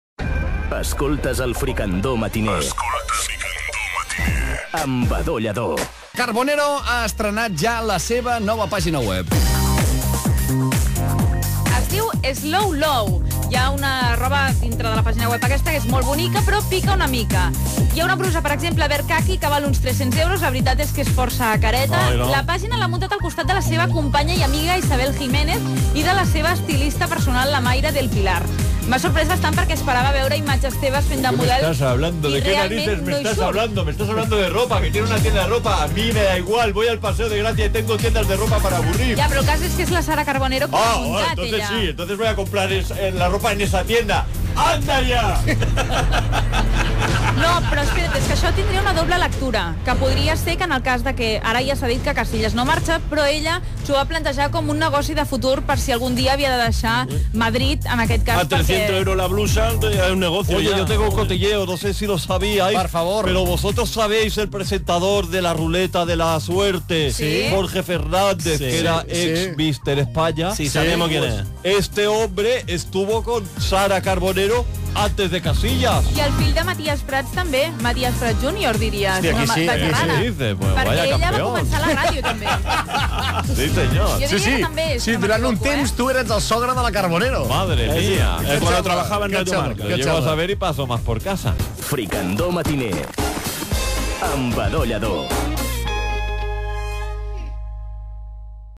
L'Alba ens parla de la nova web de Sara Carbonero
L'Alba ens parla de la nova pàgina web de Sara Carbonero on podem trobar bruses per... 300 euros.